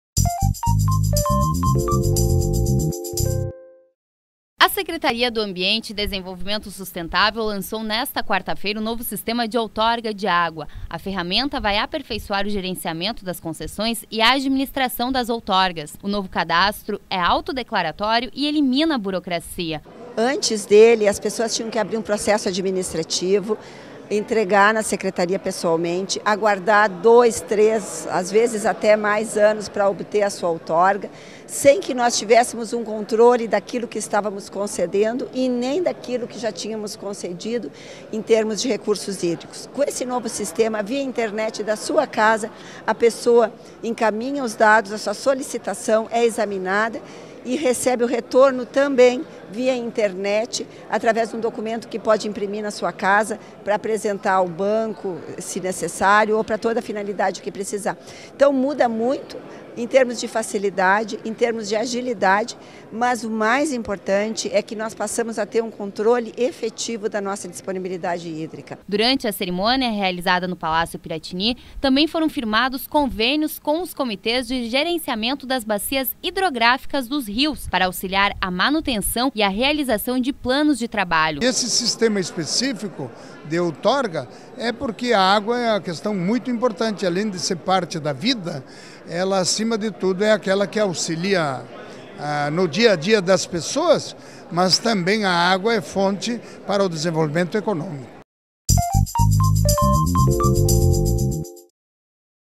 Estado lança novo sistema de autorização de uso de água
O governador José Ivo Sartori lançou, nesta quarta-feira (9), o novo Sistema de Outorga de Água do Rio Grande do Sul (Siout), em evento no Salão Negrinho do Pastoreio do Palácio Piratini. O sistema possibilitará a emissão totalmente automática e informatizada de autorização para uso da água. Ao reunir as informações relativas aos recursos hídricos de forma digital, esta nova ferramenta vai aperfeiçoar o gerenciamento das concessões e a administração das outorgas de utilização de água.